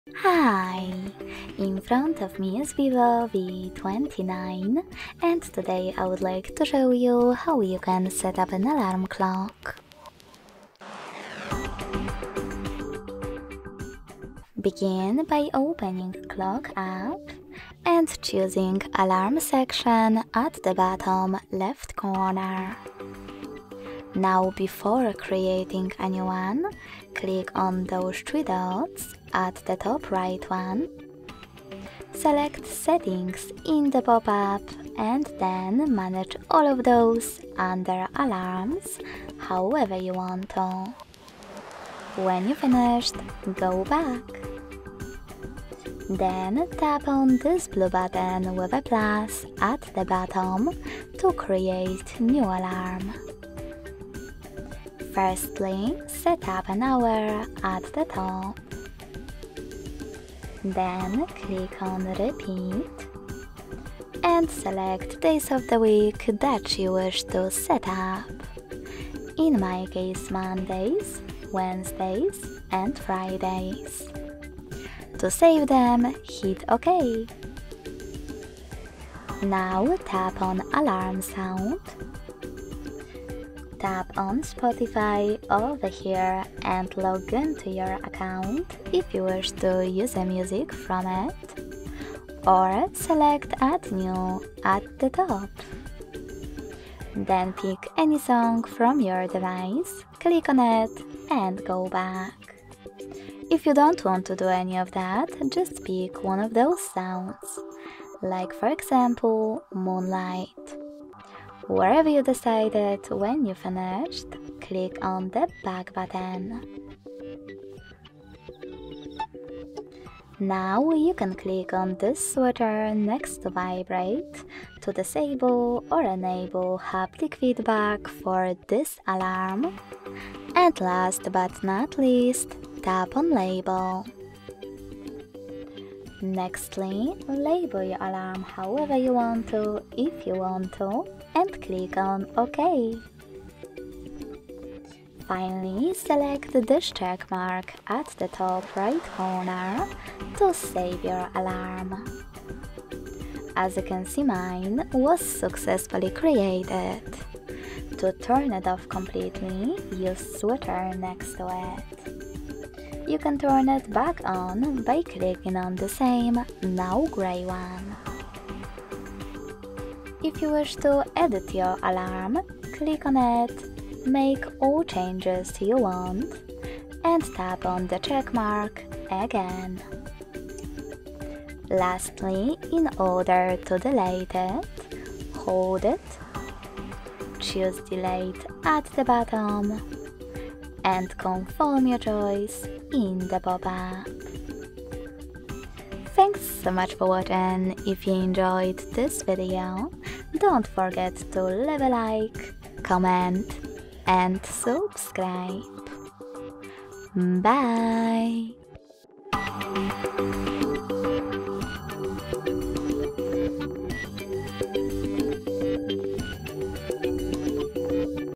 How to Set Up Alarm Clock on VIVO V29?
Find out more:
This tutorial is created to assist VIVO V29 users in setting up the alarm clock feature on their smartphones. The alarm clock functionality allows users to schedule alarms for various purposes, such as waking up in the morning or setting reminders. The step-by-step guide offers clear instructions on how to access and configure the alarm clock settings on the VIVO V29. By following this tutorial, users can effectively set alarms and manage their schedules.

Why set up an alarm clock on the VIVO V29?
Where can users find the alarm clock settings on the VIVO V29?
Can users customize alarm tones or sounds on the VIVO V29?
How many alarms can users set simultaneously on the VIVO V29?